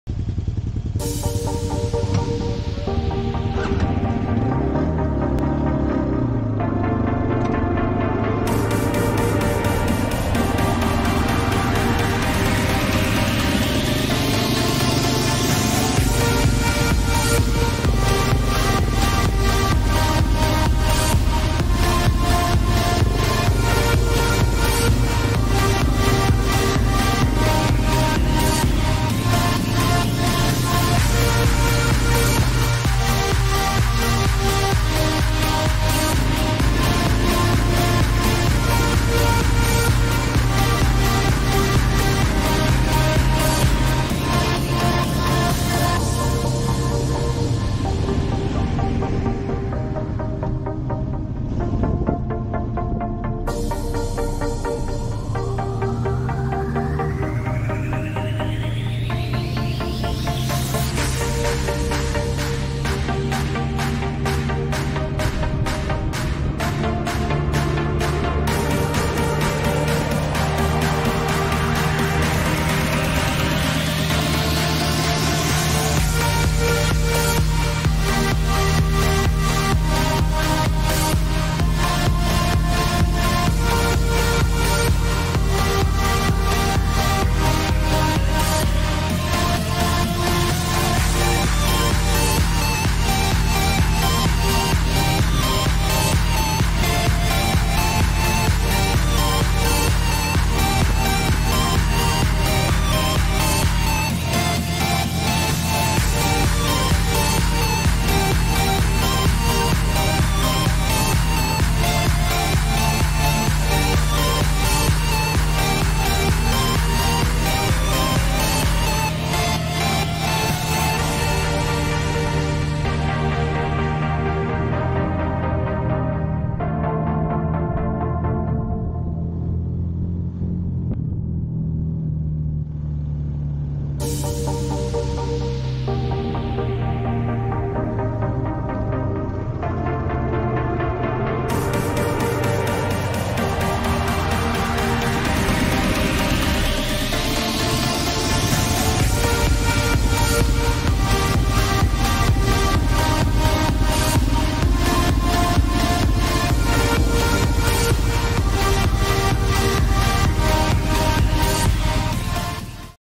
Riding ADV motorcycles over the Fontana dam.
Out group riding two KLR650s a Yamaha 250XT and a BMW 1250GSA over the top of Fontana dam in North Carolina on July. 21, 2023.  At 480' tall Fontana dam is the tallest dam in the US east of the Mississippi.  This is also a portion of the Appalachian  trail where it crosses the dam.  Shot using a Insta 360 X2